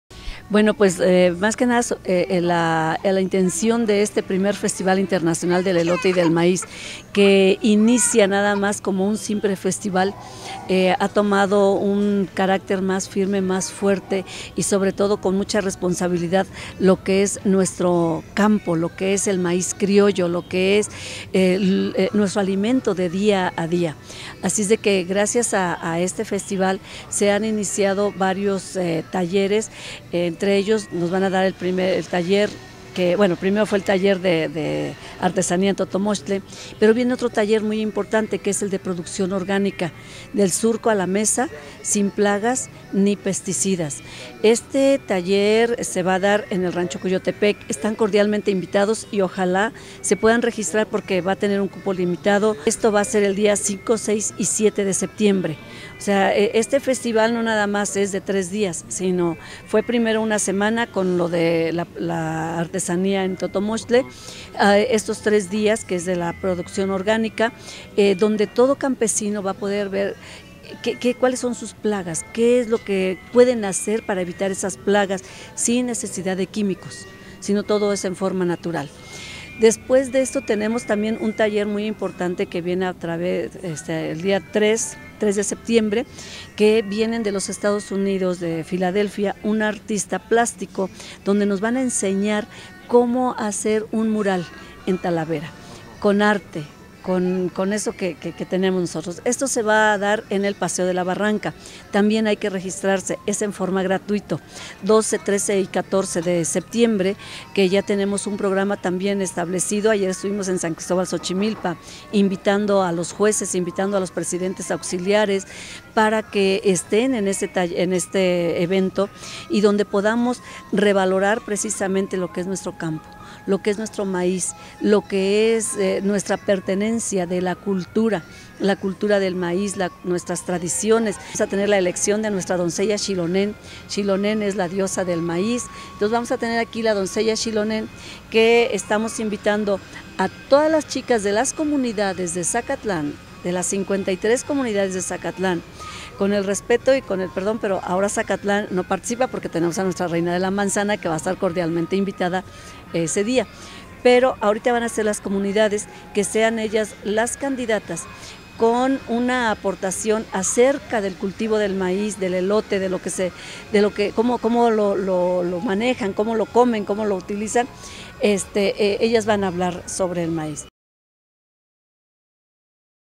El 1er. Festival Internacional del Elote y Maíz 12, 13, 14 de septiembre
Realizarán en ‪Zacatlán‬ el 1er. Festival Internacional del Elote y Maíz 12, 13, 14 de septiembre